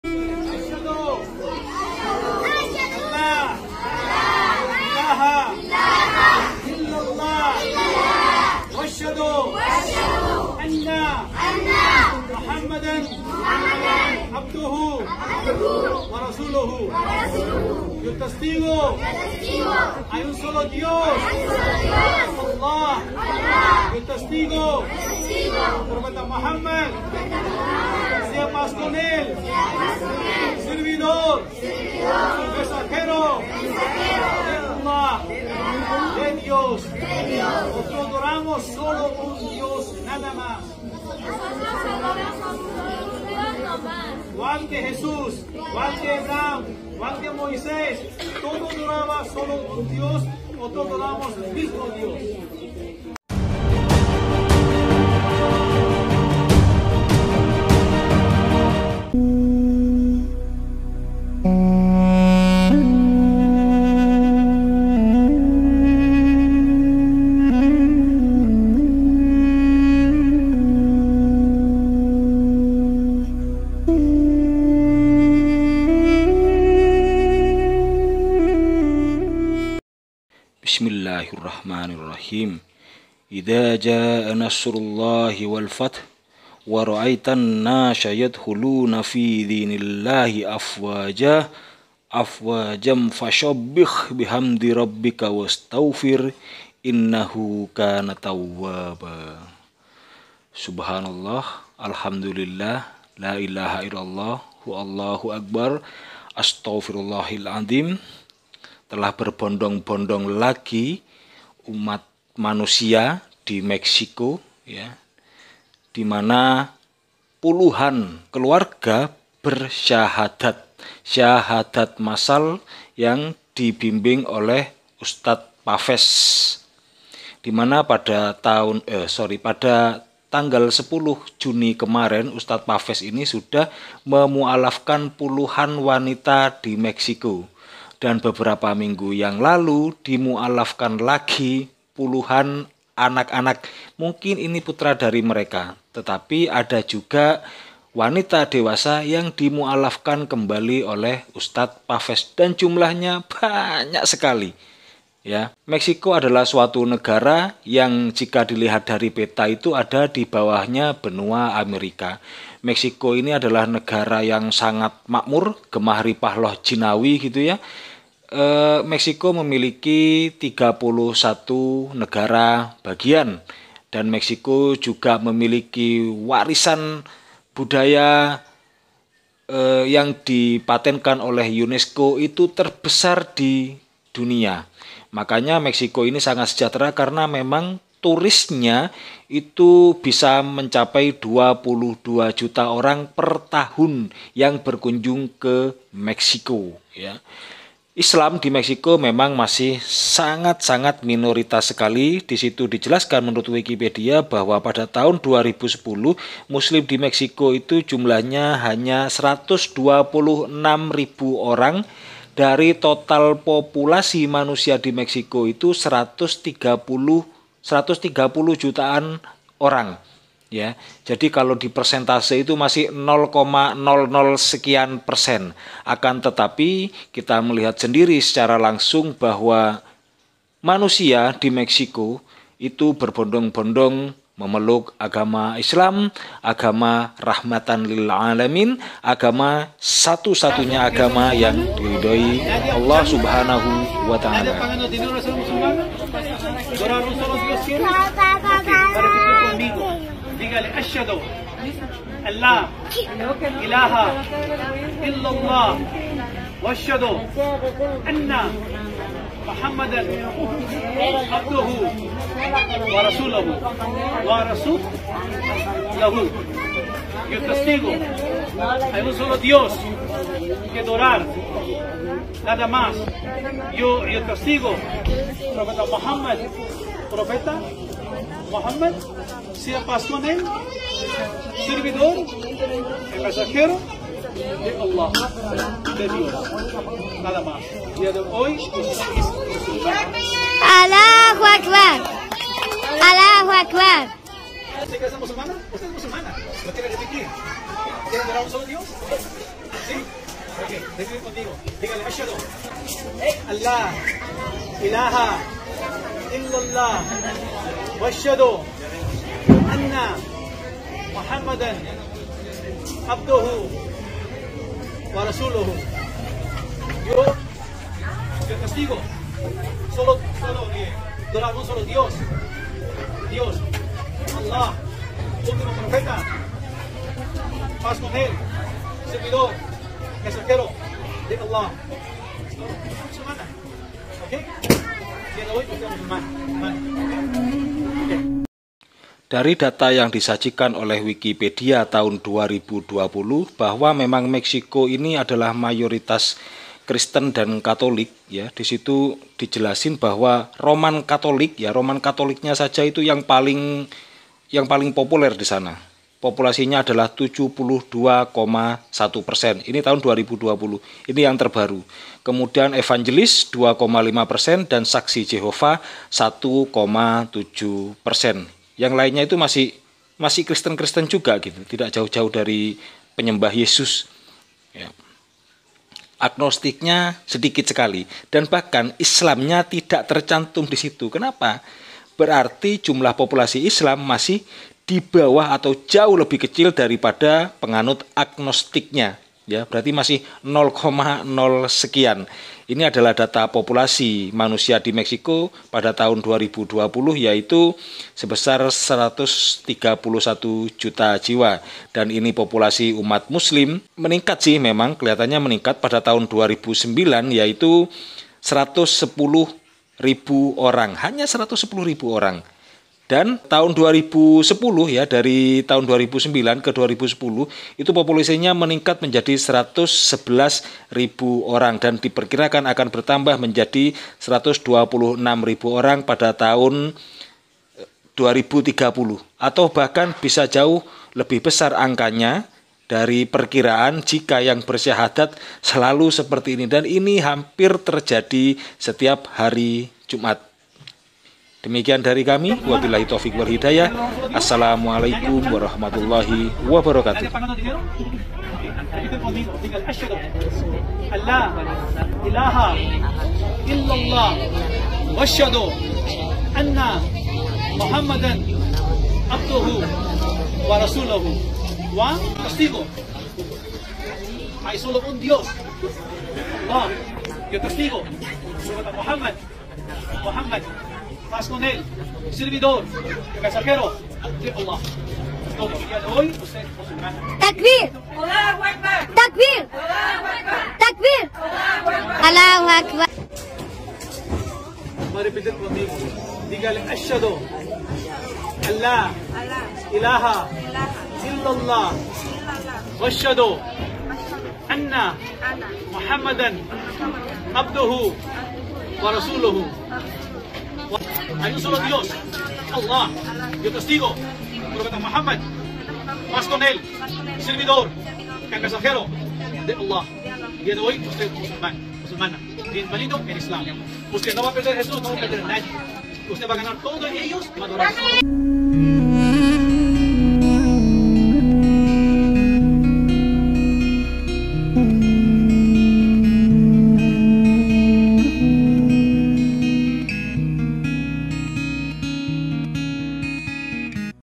PUJI TUHAN SEMAKIN BANYAK YANG MENERIMA YESUS SEBAGAI UTUSAN
MEXICO ADALAH NEGARA KAYA YANG SAAT INI ISLAM NYA MULAI BERKEMBANG DAN MESKIPUN JUMLAH MASJID DI SANA BARU ADA 10 GEDUNG TERSEBAR DISELURUH NEGARA BAGIAN MEXICO